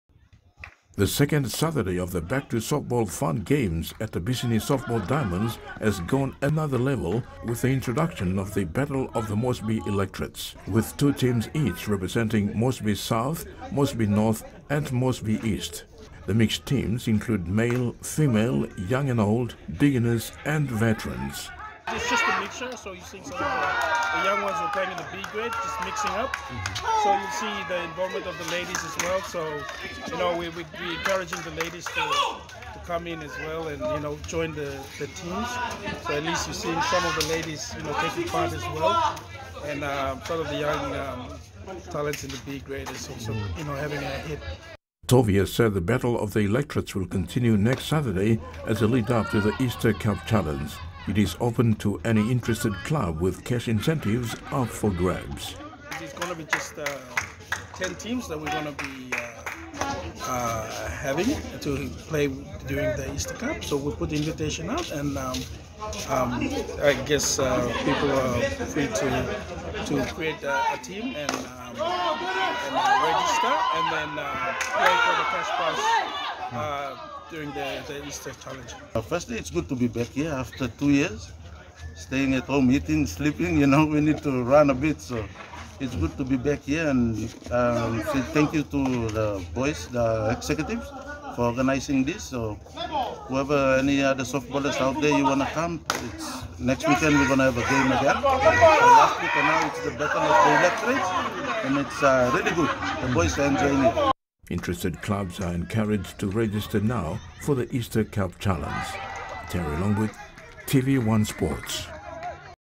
Softball Returns
After two consecutive seasons with no softball due to the impact of the COVID-19, the game is slowly making a comeback to the home of Port Moresby softball, the Bisini diamonds.

For the past two Saturdays the Port Moresby Softball Executive organized Family Fun Day activities at the diamonds to get the interest back in the softball fraternity in Port Moresby.